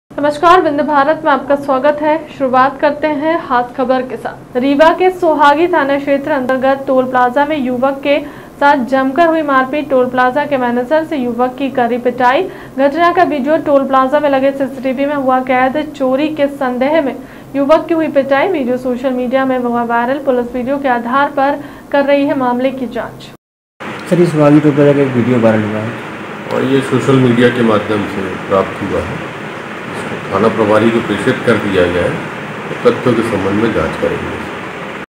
रीवा के सोहागी थाना क्षेत्र अंतर्गत टोल प्लाजा में युवक के साथ जमकर हुई मारपीट
रीवा के सोहागी थाना छेत्र अंतगर्त टोल प्लाजा में युवक के साथ जमकर हुई मारपीट टोल प्लाजा के मैनेजर ने युवक की करी पिटाई घटना का वीडियो टोल प्लाजा में लगें सीसीटीवी में हुआ कैद। चोरी के संदेह में युवक की हुई पिटाई वीडियो शोसल मीडिया में हुआ वायरल पुलिस वीडियो के आधार पर कर रही है मामले  कि जांच।